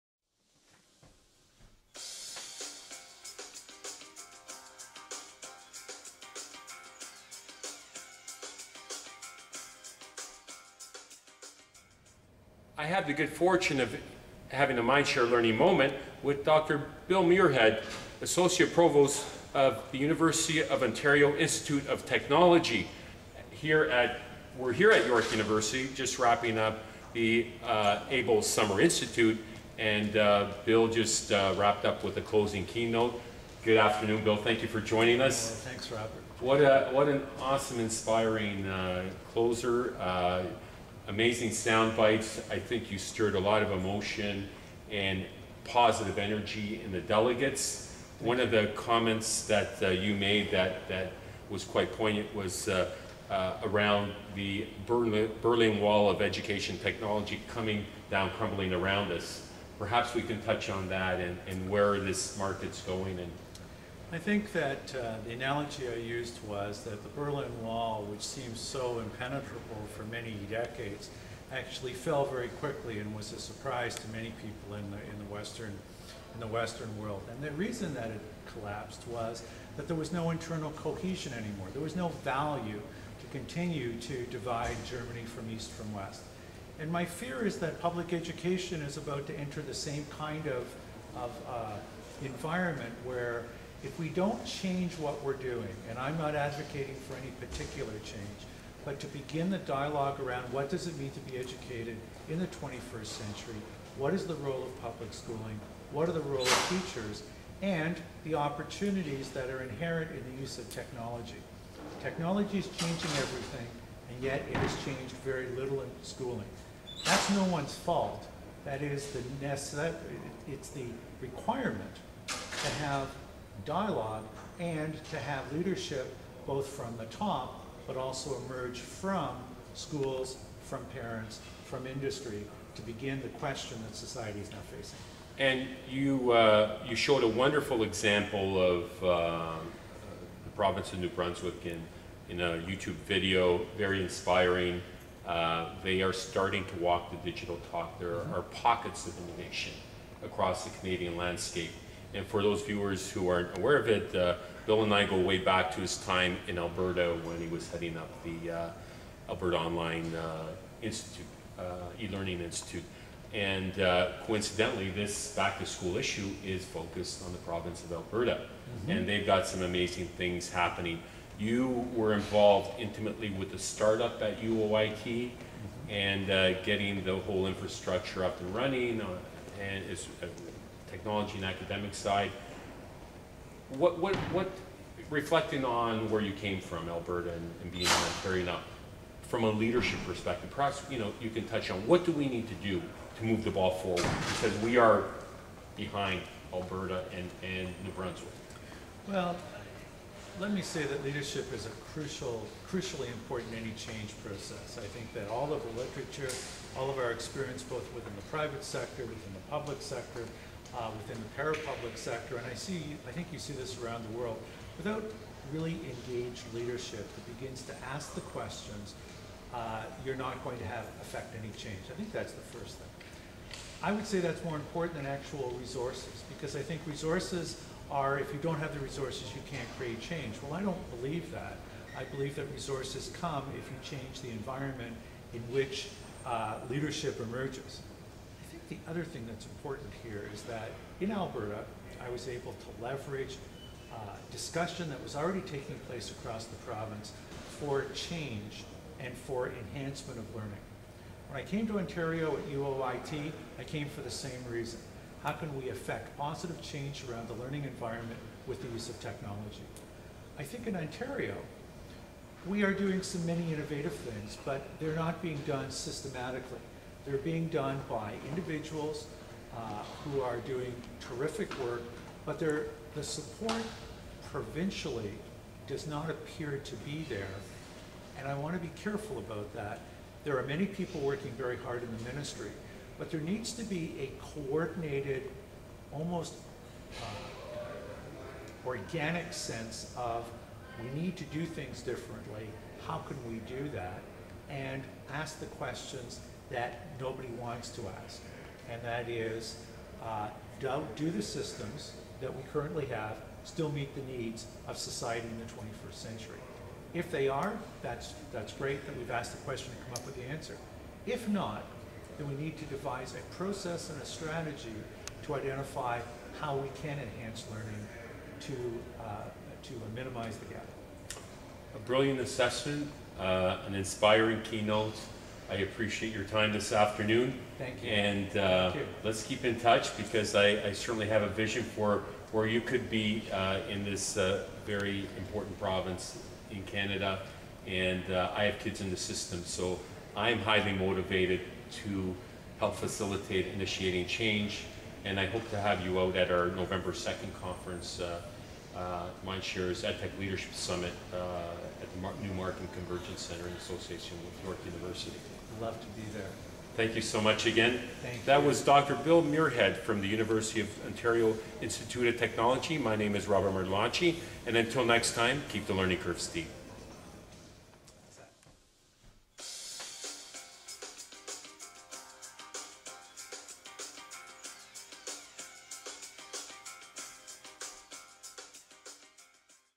Sneak Peak Interview MSL Report September Back2School Special Edition avail. Sept. 1, '10
MindShare Learning Report - September 2010 Back2School:  Blueprint for Classroom Success—Cross Canada Spotlight on Alberta Edition

Feature interview with Bill Muirhead PhD. Associate Provost Learning Technologies University of Ontario Institute of Technology.  After giving a Closing Keynote at the ABLE Summer Institute Conference at York University.